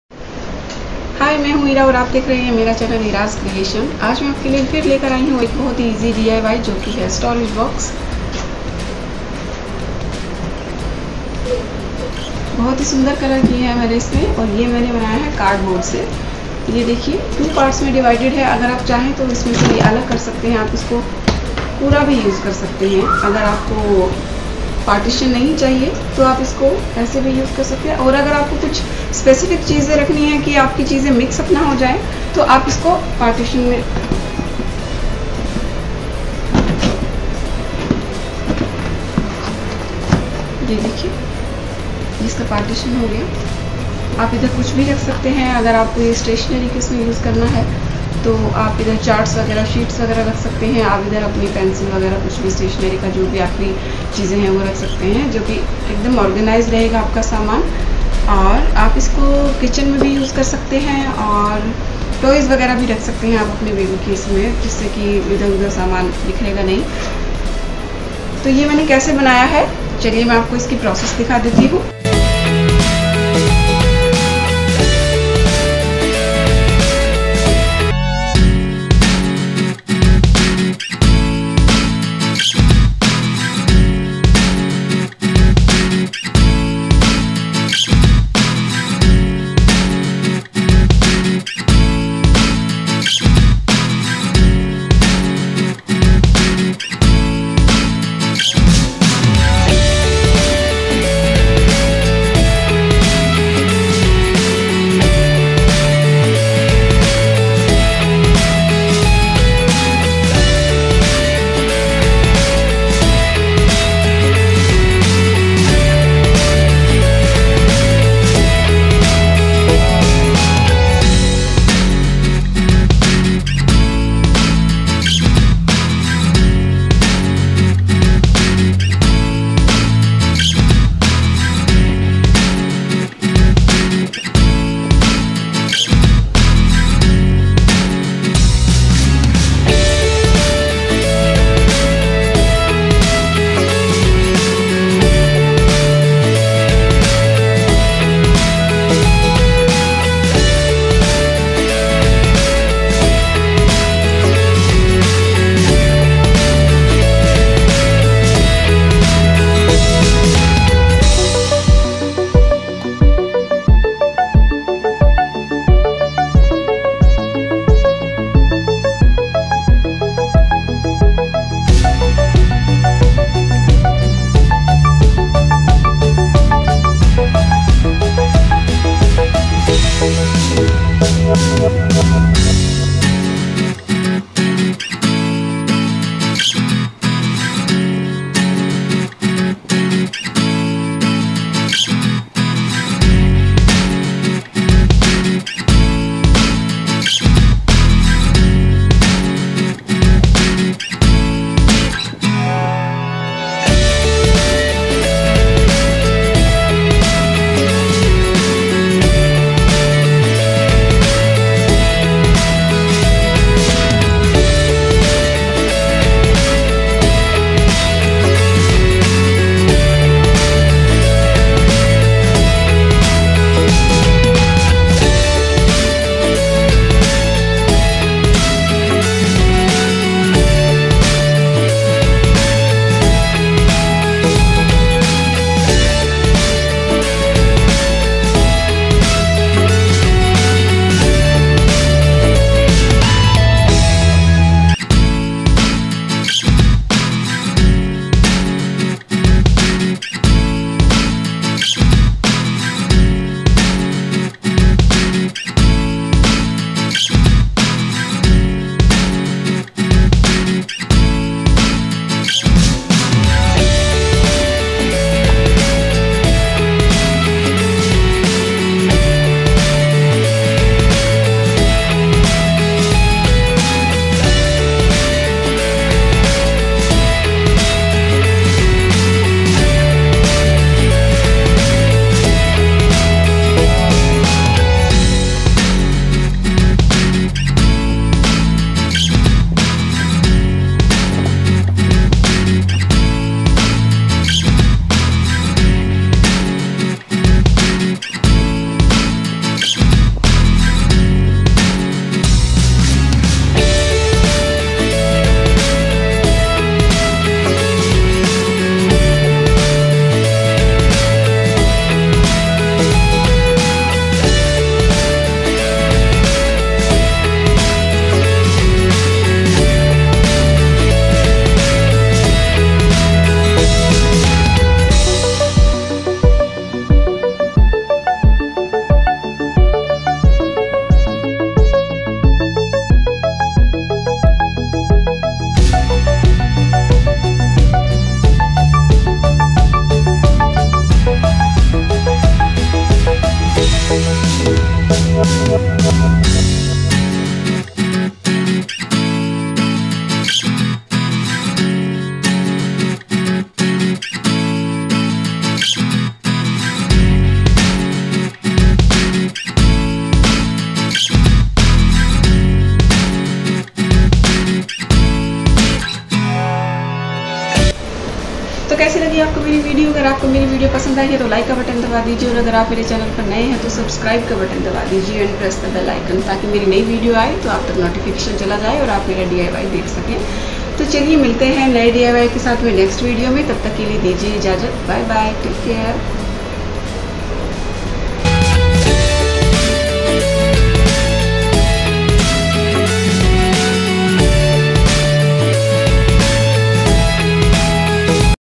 How to Make Storage Box Out of CardBoard | DIY | Best Out of Waste Idea | Home Decor | Hand Made
Today I am going to share tutorial for How to Make Storage Box Out of CardBoard.

Materials needed

1. Card Board
2. Glue Gun.
3. Tissue Paper
3. POP
4. Feviciol & Water
5. Fabric Colours
6. Duct Tape
7. Scissor

Storage Box
Hand Made

Try this at home and let me know your comments and suggestions . Happy crafting ?